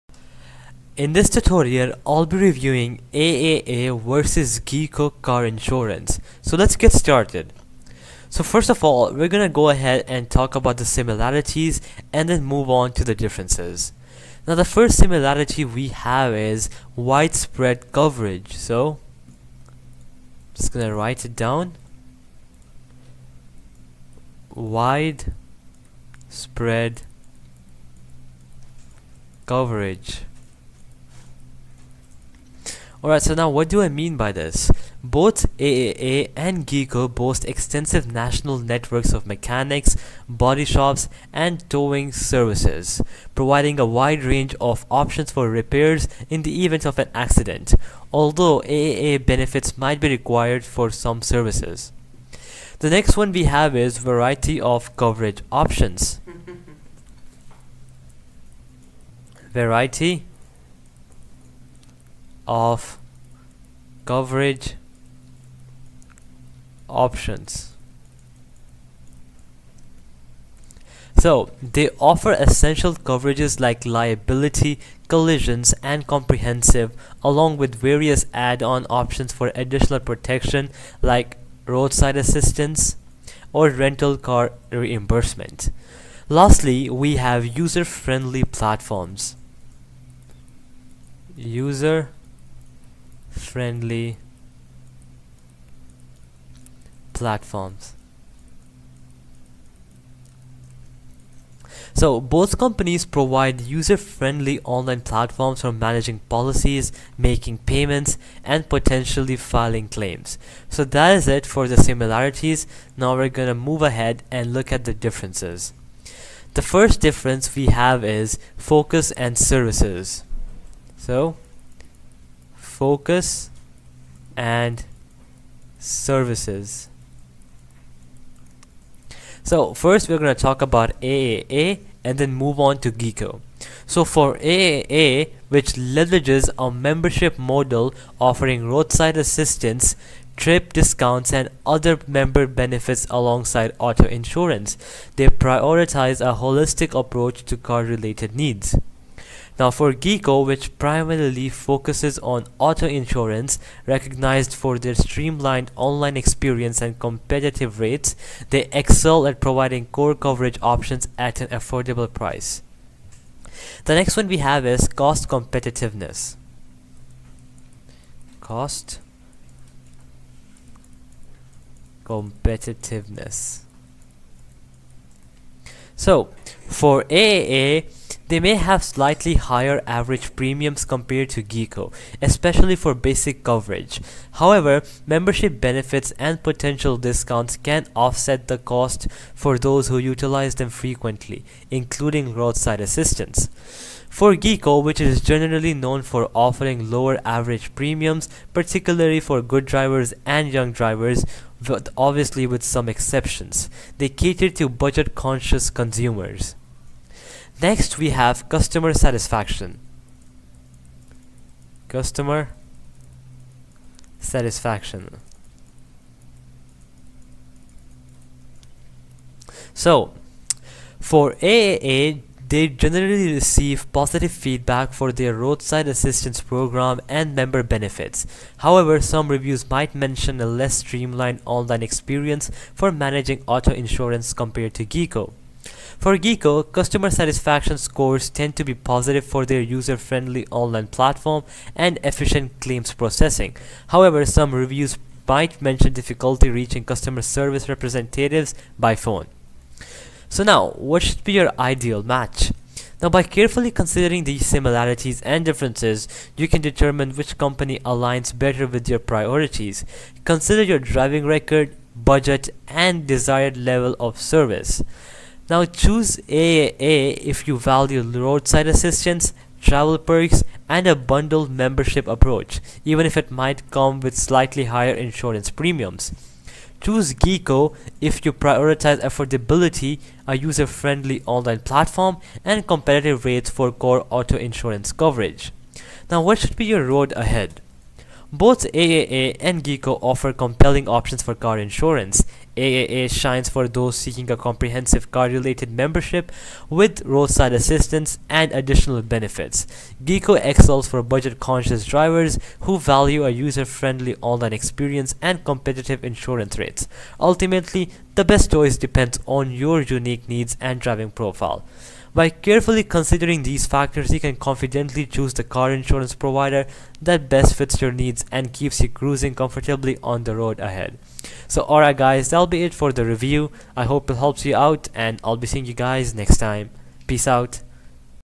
AAA Vs Geico Auto/Car Insurance | Is AAA Better/Cheaper Than Geico? ( Roadside Assistance)
AAA (American Automobile Association) is a federation of motor clubs throughout North America. While AAA is widely known for its roadside assistance and travel services, it also offers a range of insurance products, including auto insurance.

Geico (Government Employees Insurance Company) is a leading American auto insurance provider known for its competitive rates and extensive advertising. Geico was founded in 1936 and initially targeted government employees and military personnel. Today, it offers insurance to a broad audience and is a subsidiary of Berkshire Hathaway